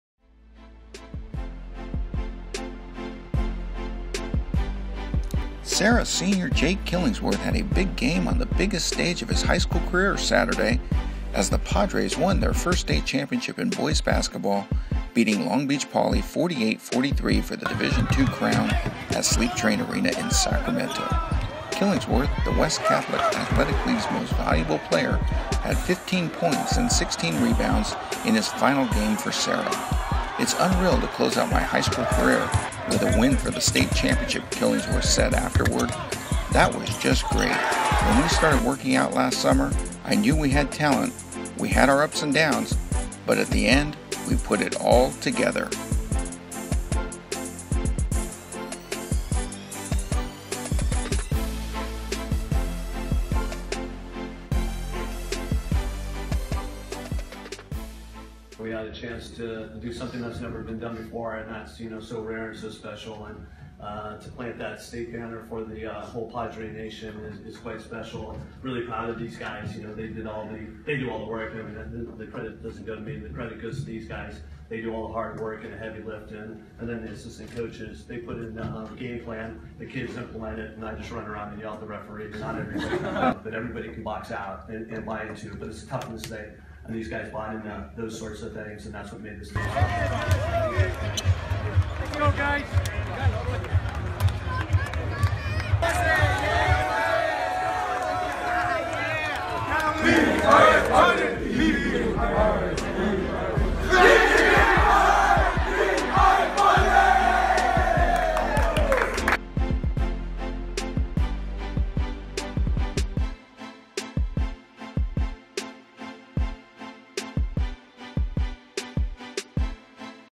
WATCH: Recap of Serra's victory Saturday over Long Beach Poly in CIF Division II state championship
WATCH: Recap of Serra's victory Saturday over Long Beach Poly in CIF Division II state championship boys basketball game

By: Darren Sabedra
Published on: March 27, 2016